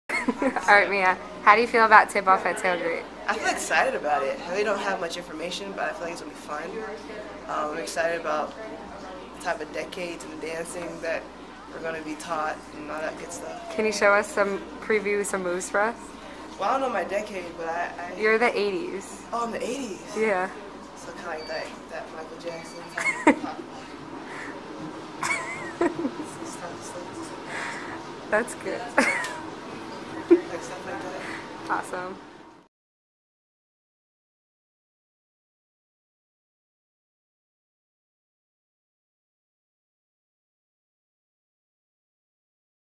Tip Off at Tailgreat!
Come join the Penn State men's and women's basketball teams as they host Tip Off at Tailgreat in the BJC at 5 p.m. on Saturday, October 30th.